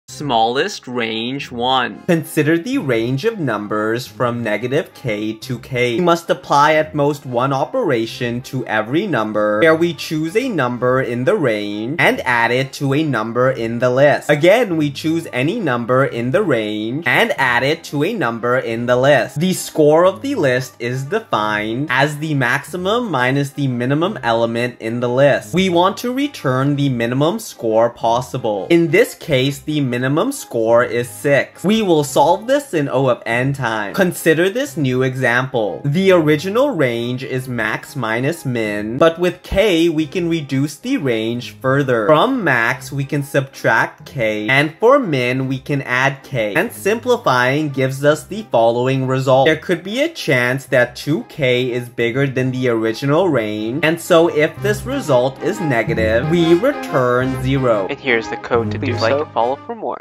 LeetCode#908 Smallest Range I - Python
Solving LeetCode problem #908 Smallest Range I in Python